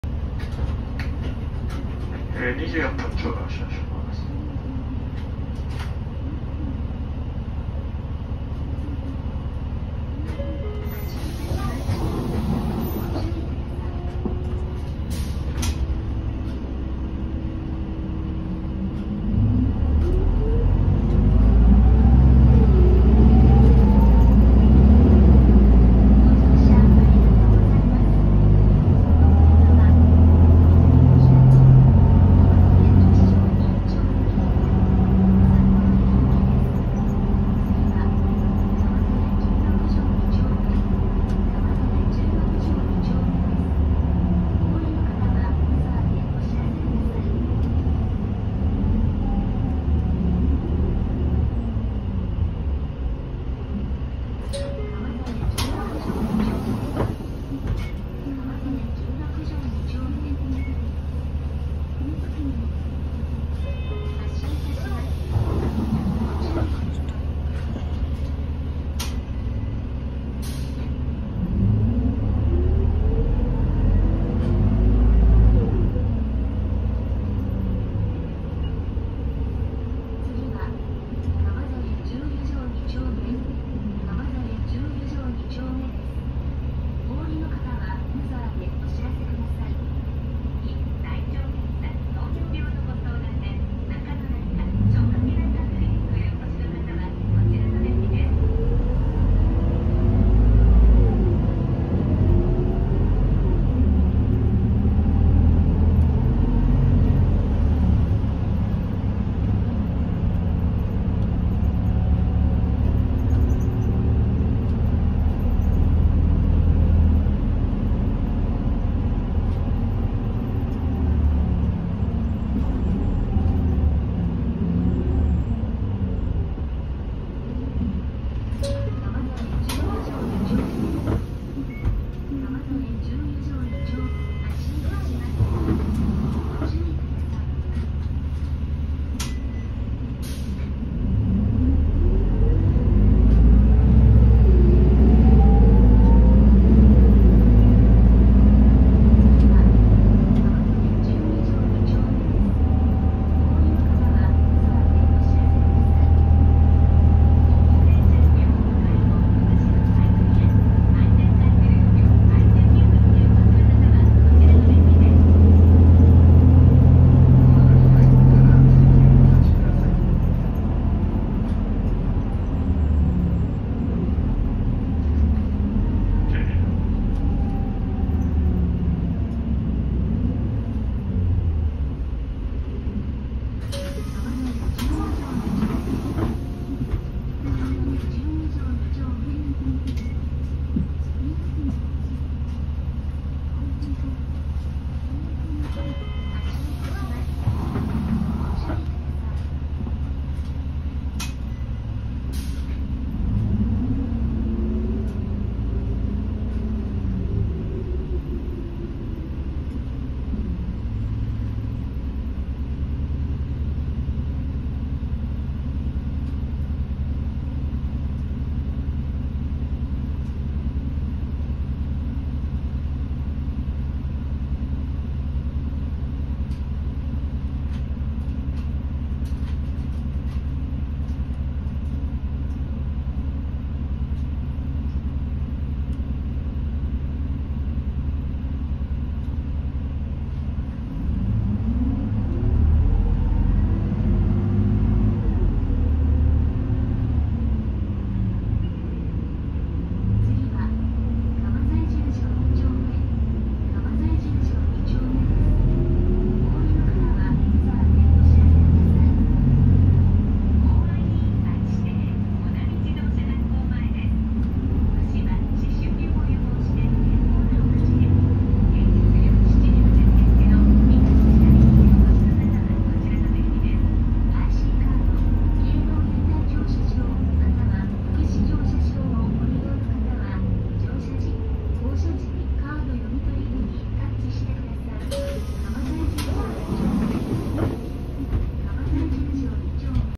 ［17年式/川沿所属］じょうてつバス 2PG-LV290Q2 車内風景
［諸元］
事業者名…じょうてつバス
所属先…川沿営業所
車両番号…札幌200か4891(川 68)
メーカー…いすゞ
形式…2PG-LV290Q2
年式…2017
元事業者…
収録区間…南55 札幌駅行き 硬石山→川沿10-2

5741が走るかもとの情報を聞きつけて行ってみましたが4891が運用に。
日野メインの川沿では少数派ないすゞ車となっています。
藻岩の同形式は撮影済みでしたが川沿のこちらは未撮影だったのでこのタイミングで回収。